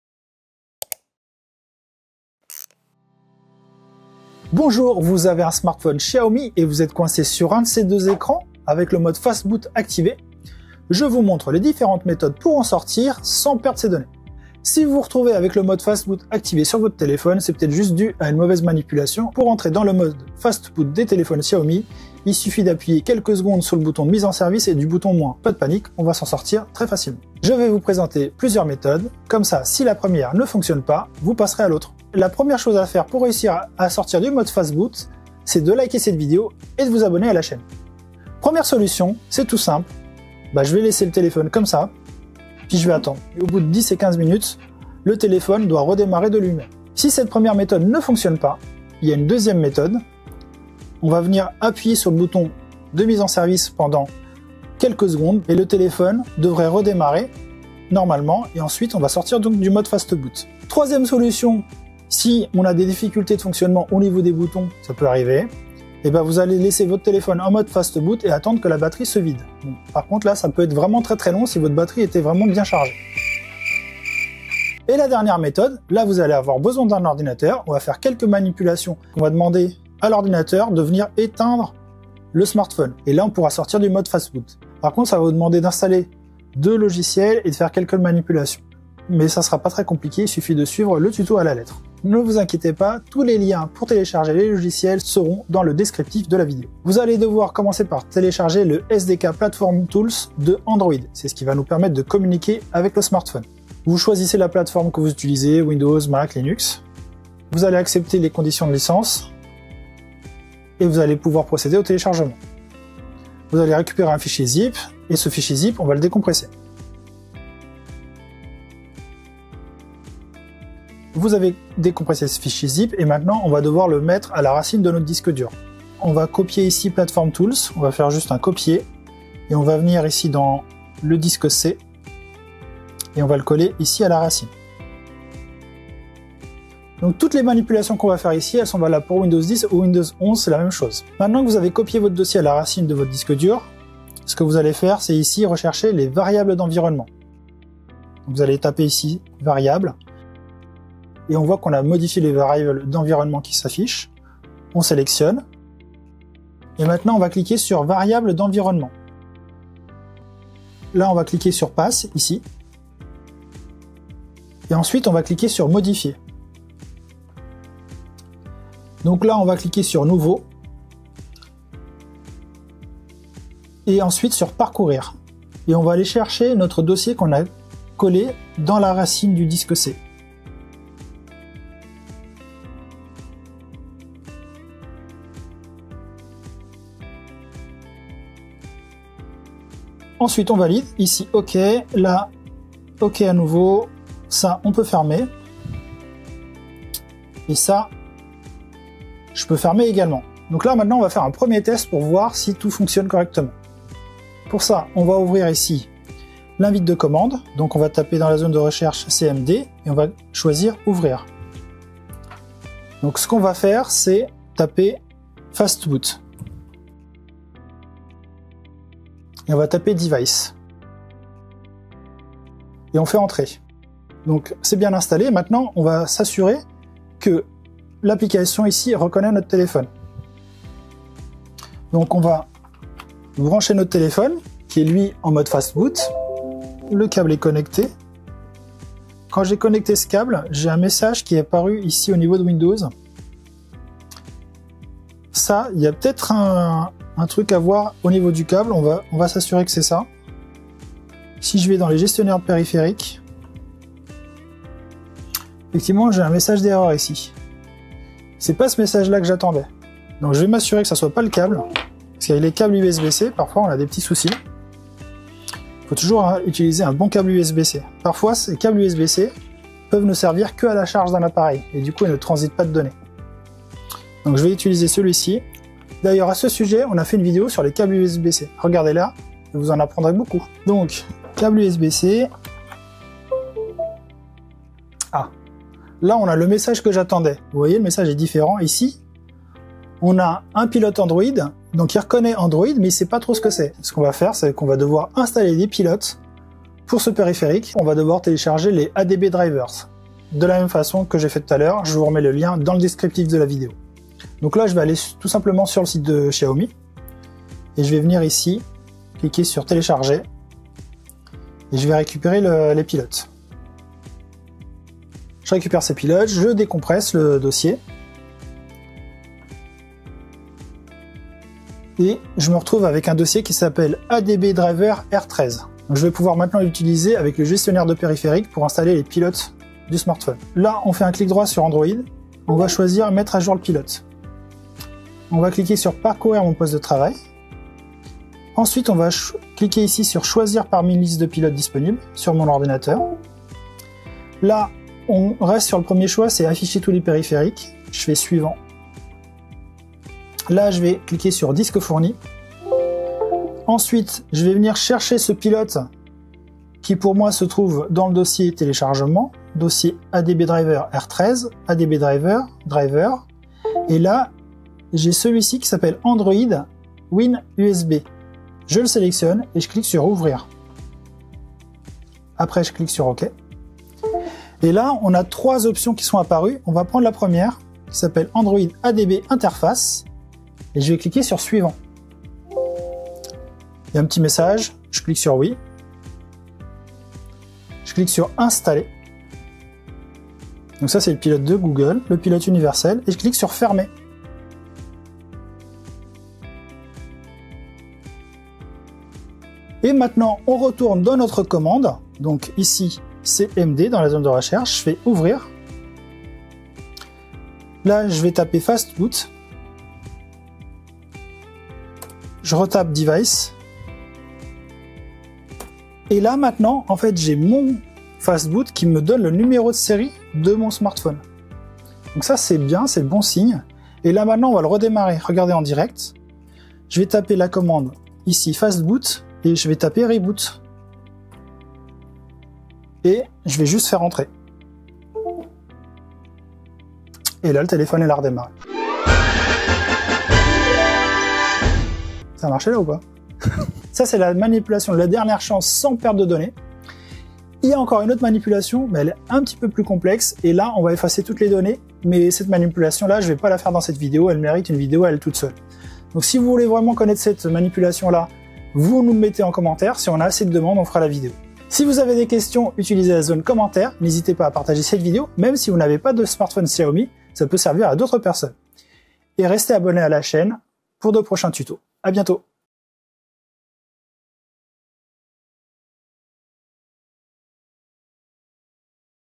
Sortir du Fastboot en 30 secondes ! (Astuce Xiaomi)
Votre téléphone Xiaomi est bloqué sur un écran qui affiche FASTBOOT et vous ne pouvez plus rien faire, dans cette vidéo je vous donne plusieurs solutions pour en sortir sans perdre vos données.
Voici 4 solutions efficaces pour sortir du mode Fastboot sur un smartphone Xiaomi :

✅ 1. Attendre sans toucher à son smartphone.
Posez votre téléphone sur votre table et attendez 10 à 15 minutes sans y toucher. Votre smartphone doit redémarrer tout seul sur la page d'accueil.

✅ 2. Redémarrage forcé.
Appuyez sur la bouton Power (alimentation) pendant plusieurs secondes jusqu’à ce que le logo Xiaomi apparaisse.

✅ 3. Attendre que la batterie se vide.
Posez votre téléphone et attendez que la batterie se vide complètement pour que votre téléphone se coupe. Si ces trois solutions ne fonctionnent pas il en reste une autre en utilisant un PC.

✅ 4. Utilisez un ordinateur.
Connectez votre smartphone en USB sur l'ordinateur pour sortir du mode Fastboot.
Pour cette solution vous allez  devoir installer un logiciel sur votre PC et télécharger des pilotes.